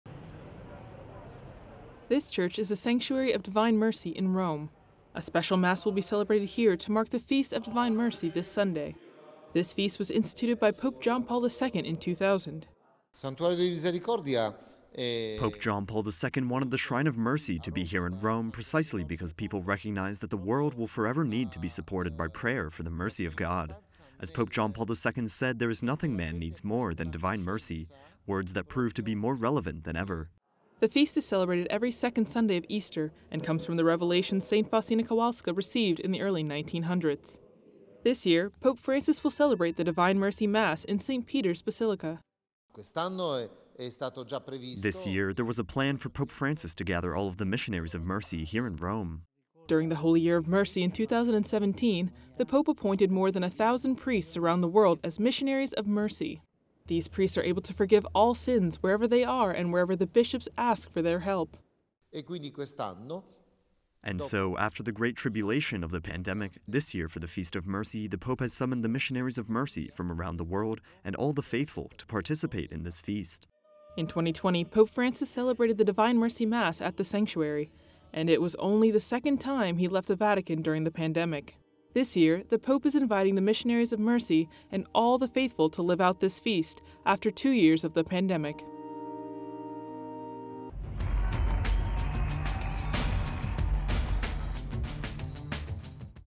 DivineMercy Sunday “Nothing man needs more than Divine Mercy”
This Sunday is the feast of DivineMercy. PopeFrancis will celebrate Mass at 10am in St. Peter's Basilica.

This Church is the Sanctuary of Divine Mercy in Rome.
A special Mass will be celebrated here to mark the Feast of Divine Mercy this Sunday.
This feast was instituted by Pope John Paul II in 2000.